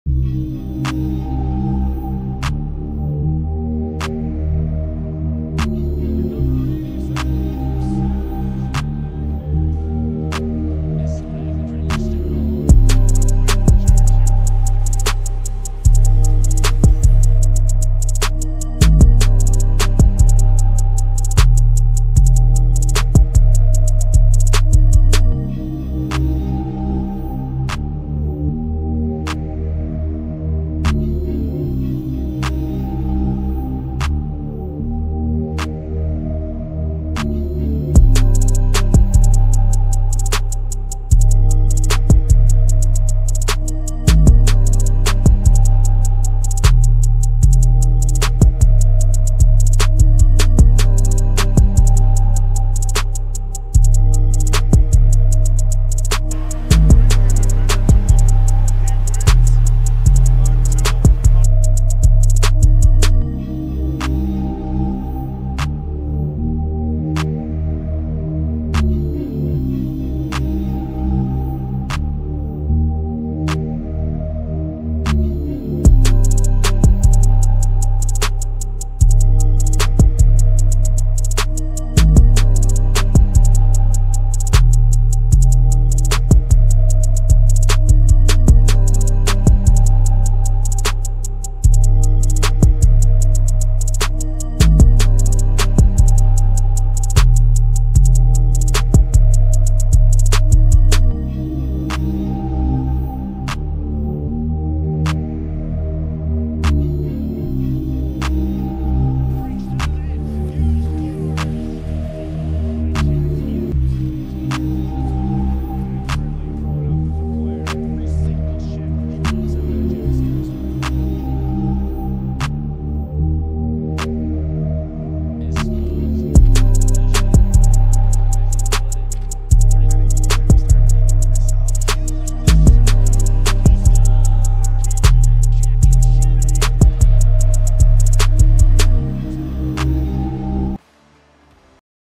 Jack Hughes - All in, Jack hughes best highlights in 3 minutes
Who should I do next? Remember to check out Pythonick1's awesome content and NotMando.